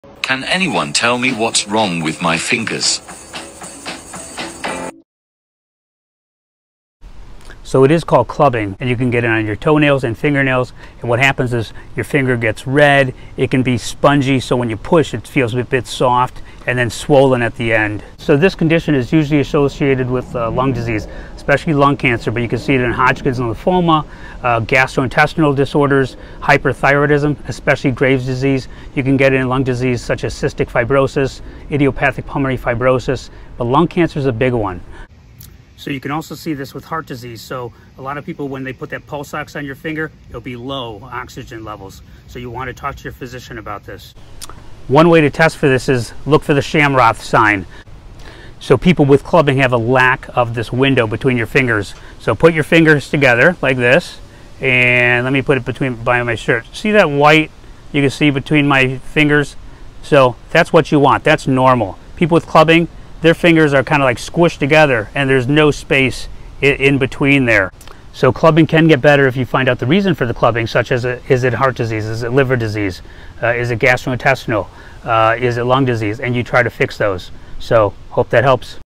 SEE YOUR DOCTOR IF YOU HAVE THIS NAIL ABNORMALITY!
Nail clubbing can be a sign of a very serious disease such as lung cancer, lung disease, heart disease, hyperthyroidism, gastrointestinal disorders. Many of these patients are found to have low oxygen levels as well. Here I will show you some of the characteristics to look for and a test you can do to see if you may have clubbing of your nails.￼

⭐️DOCTOR VINNIE⭐️
Board-certified in Family Medicine
Board-certified in Sports Medicine